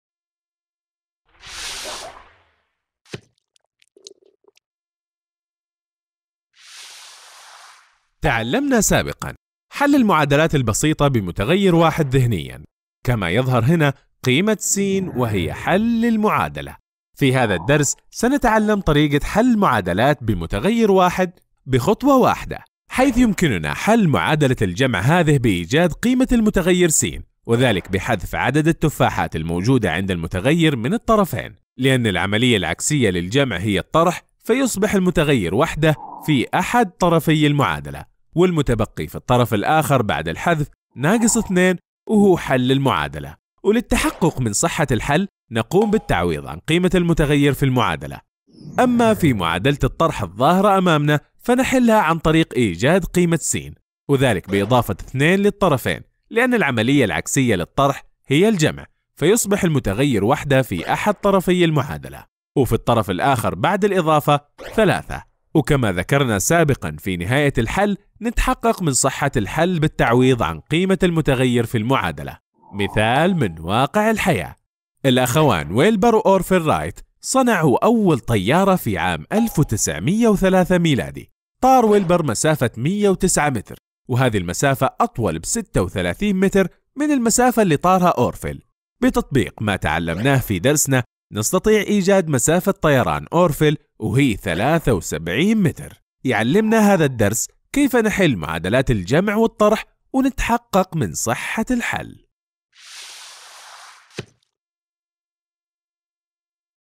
تشويقة : معادلات الجمع والطرح
فيديو تشويقي لمادة الرياضيات لدرس معادلات الجمع والطرح  لوحدة الجبر : المعادلات الخطية والدوال للصف 1متوسط   للفصل الدراسي الأول